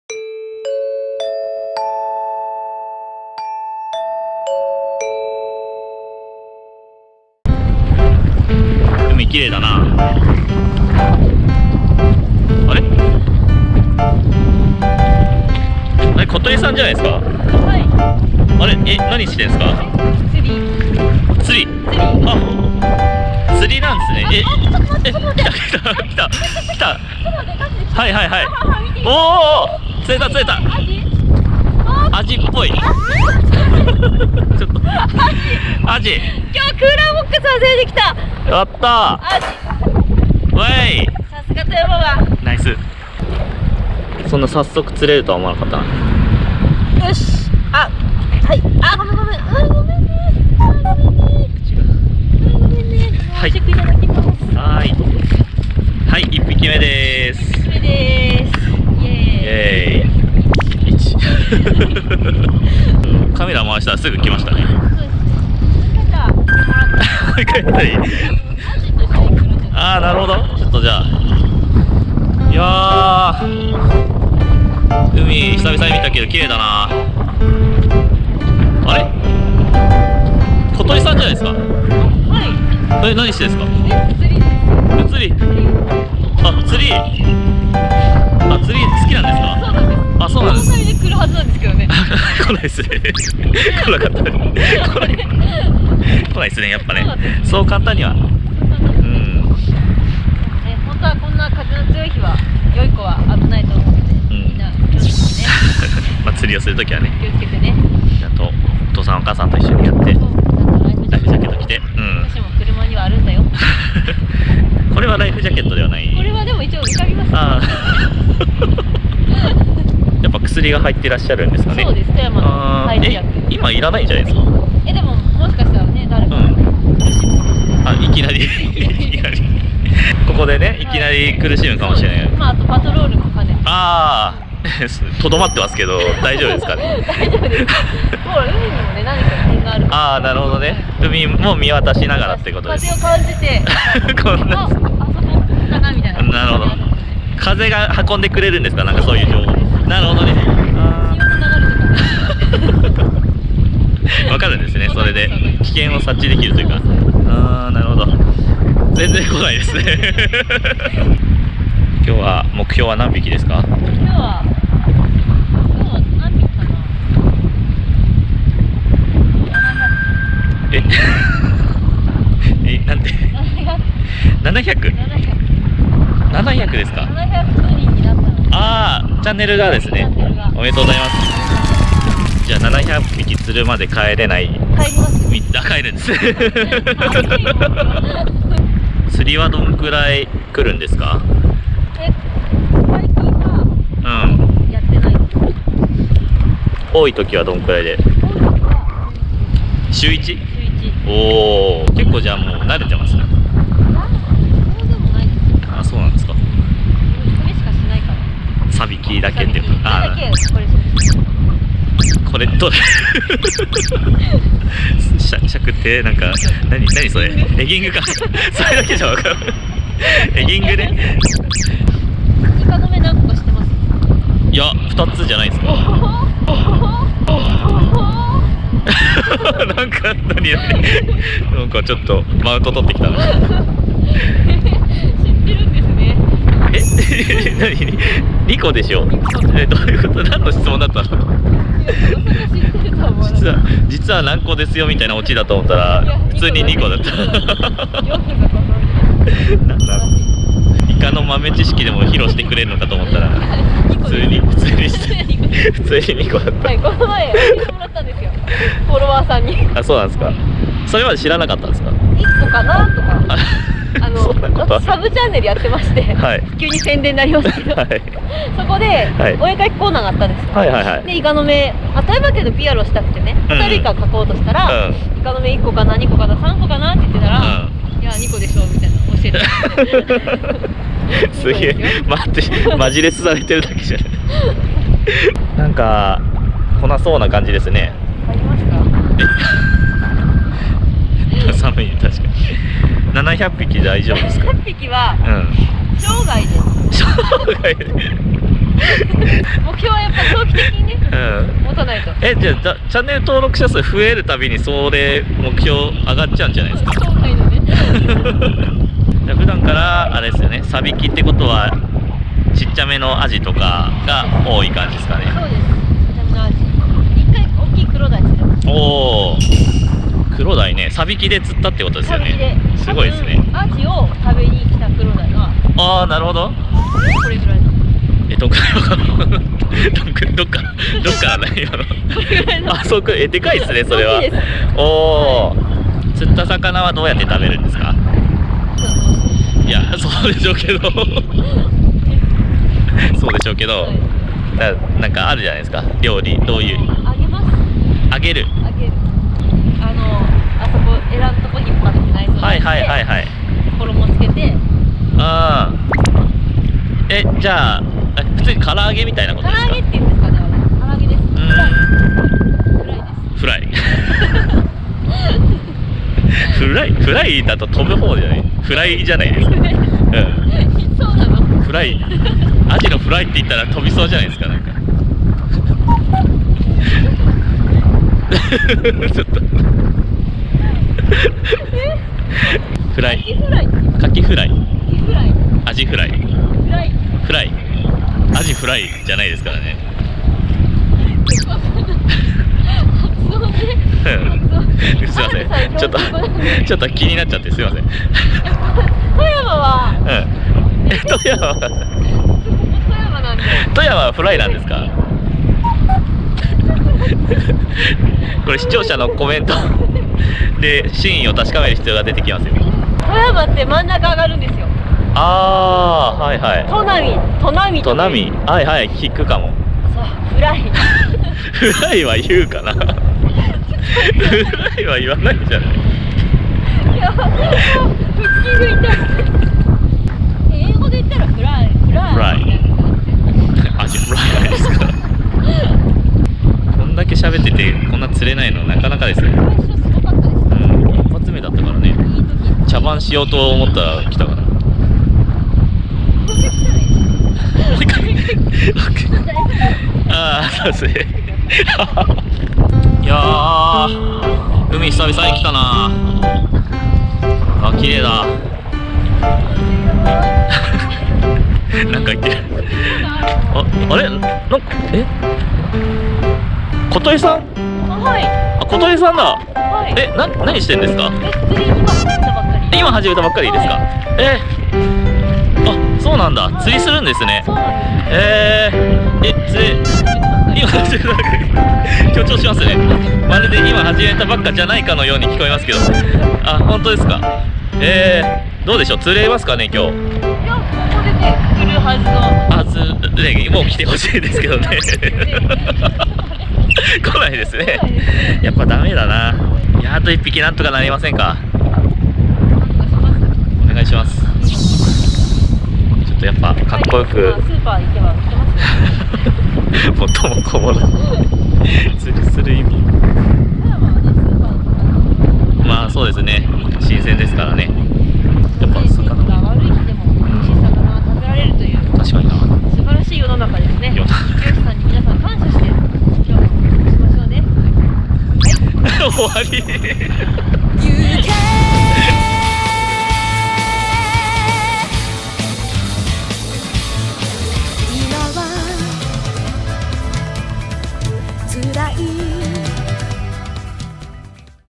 【釣り動画】三日月ことりが富山湾で釣りに挑戦…！！【キットムーン】
※とても強風の日の撮影だった為、風の音がかなりうるさいです。ごめんなさい。(R)

本当にごめんなさい（ことり）